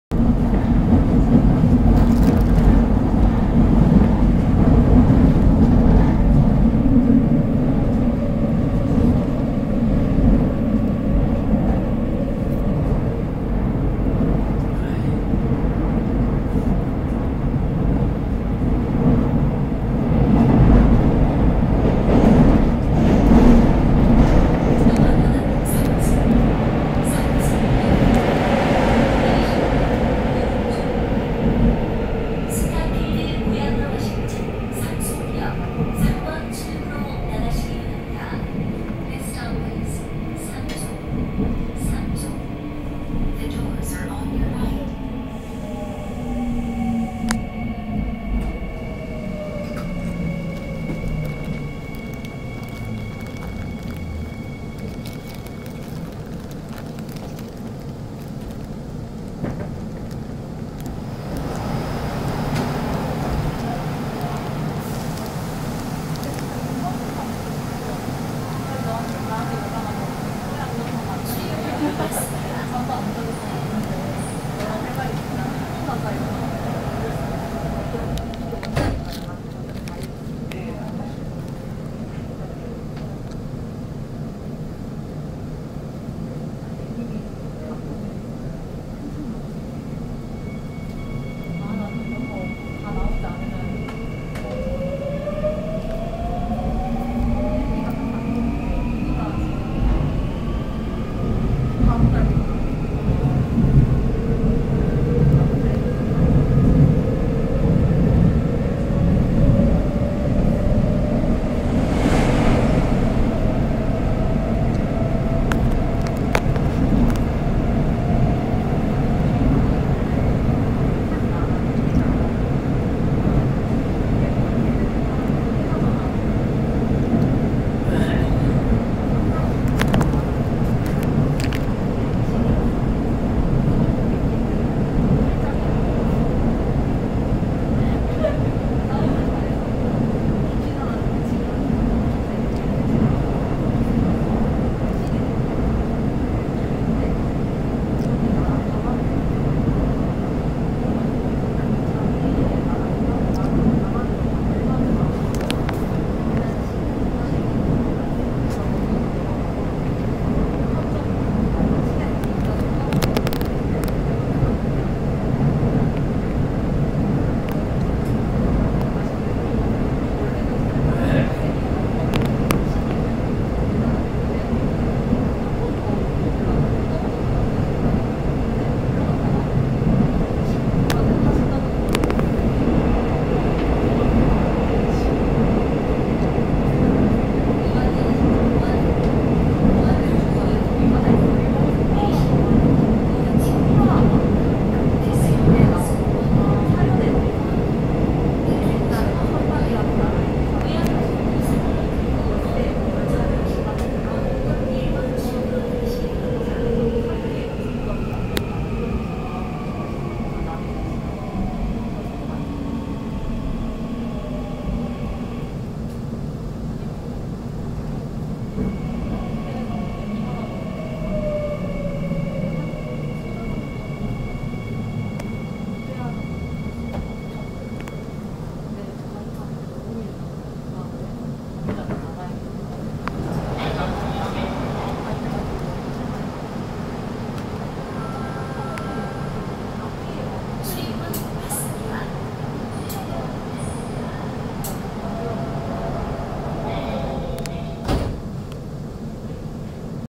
Lots of Miniature Metal Canned Foods-01
Sun 13rd Jun 2021(AH 1442 Dhul-Qadah).
I bought Lots of Miniature Metal Canned Foods this Monday.
And this is a part of Seoul Metro No 3 Line from Sam Song(318) to Won Heung(317)...